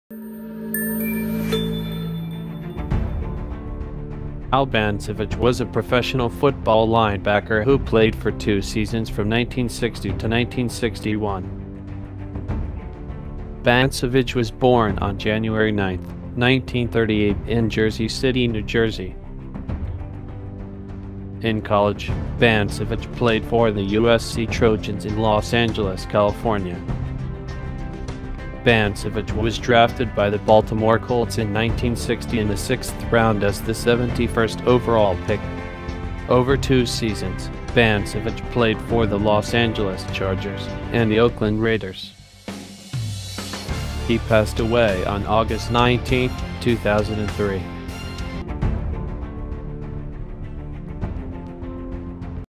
Al Bansavage: Football Linebacker
Al Bansavage was a professional football linebacker who played for 2 seasons from 1960 to 1961.

In college, Bansavage played for the USC Trojans in Los Angeles California.

Professionally, Bansavage played for the Los Angeles Chargers, and the Oakland Raiders.